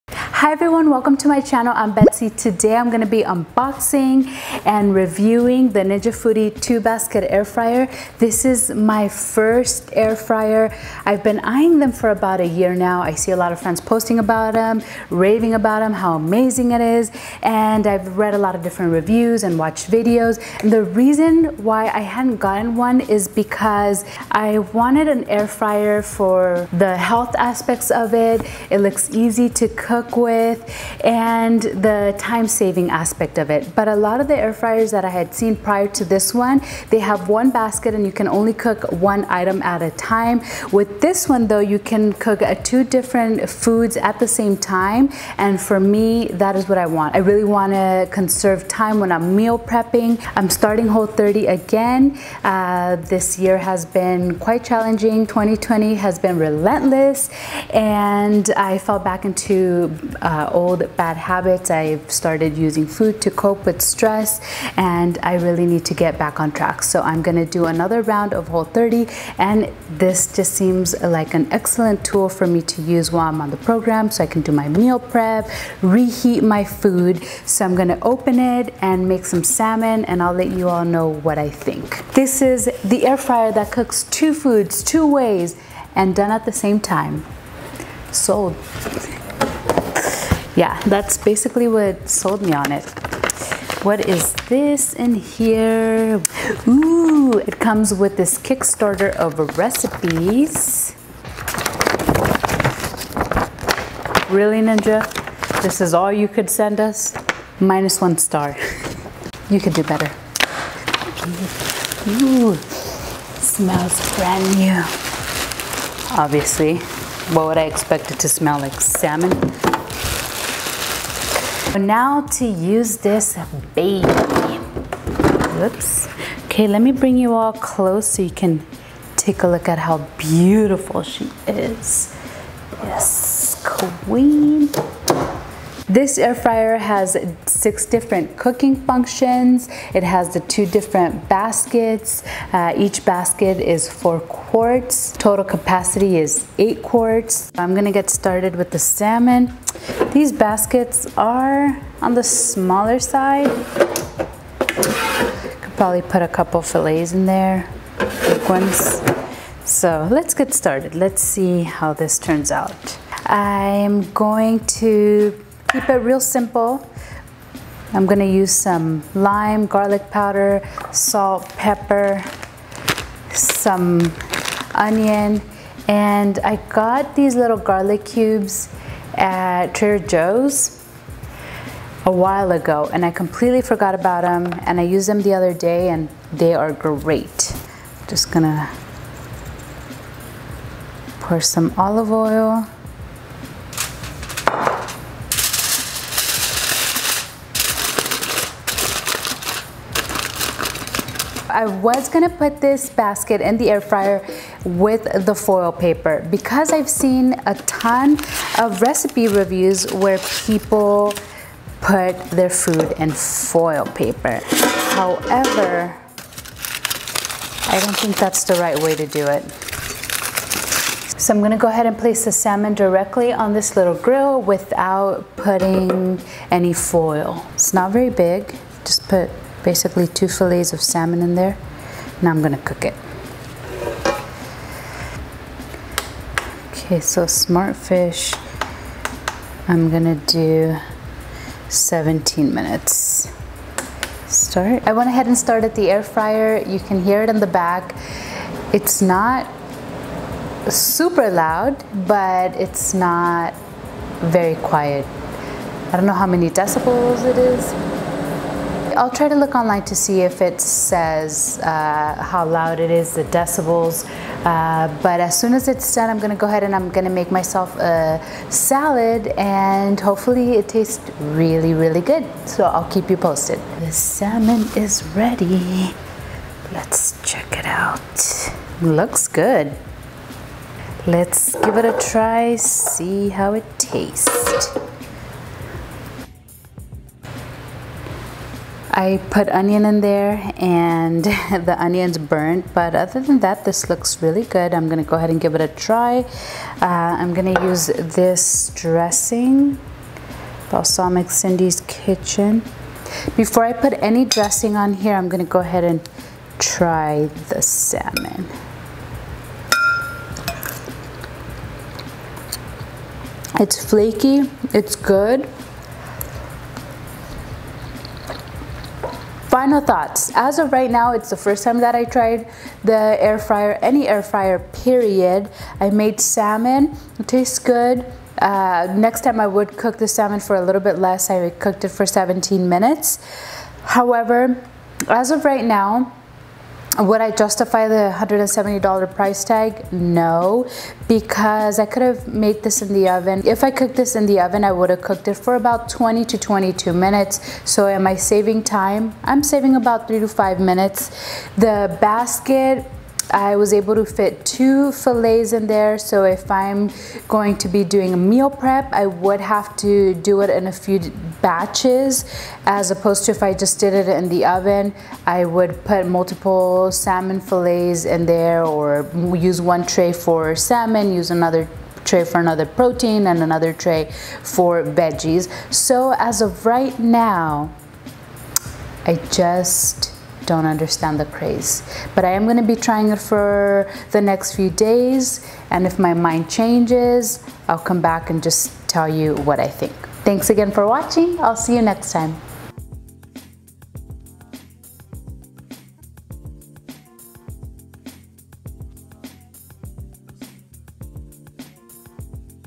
Ninja Foodi 2 Basket Air Fryer First Impression
This is my first air fryer, in the past year I've seen a lot of people posting about how much they love their air fryer. I've been looking for the right one for the past few months but the reason why I held back is that most air fryers I saw, you need to cook your meals in batches. The reason why I went with this one is that it has two baskets and I can make two different foods at the same time.

The main reason I went with the Ninja Foodie 2 basket air fryer is that:
The first air fryer lets you cook two foods, two ways, at the same time, not back to back like a traditional single-basket air fryer.